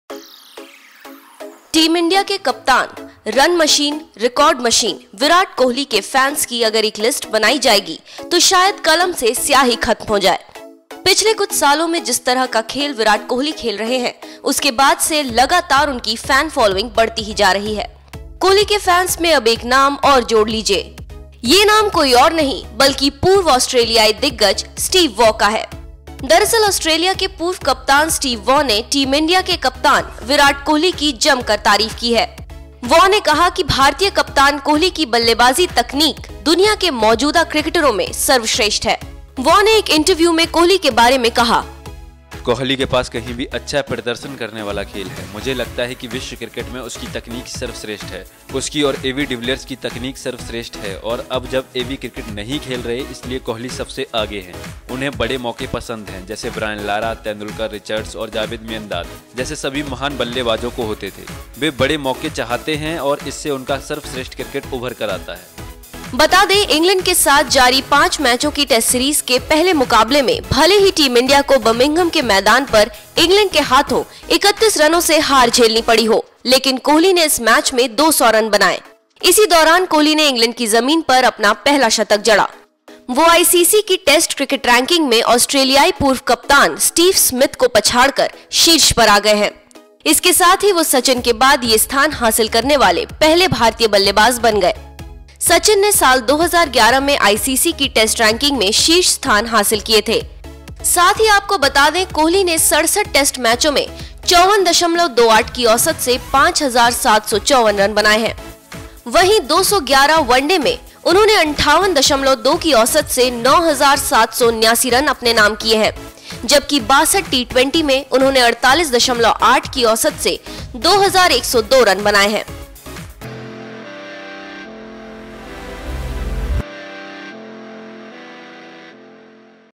Steve Waugh ने बांधे Kohli की तारीफों के पुल, बताया World का सर्वश्रेष्ठ बल्लेबाज
टीम इंडिया के कप्तान विराट कोहली का दीवान कौन नहीं है... जिस तरह से वो खेल को खेलते है लाज़मी है उनकी दीवानगी लोगों में बढ़ेगी ही... ऐसे में अब एक और महान खिलाड़ी कोहली के दीवाने बन गए है... जानने के लिए देखिये हमारी ये रिपोर्ट...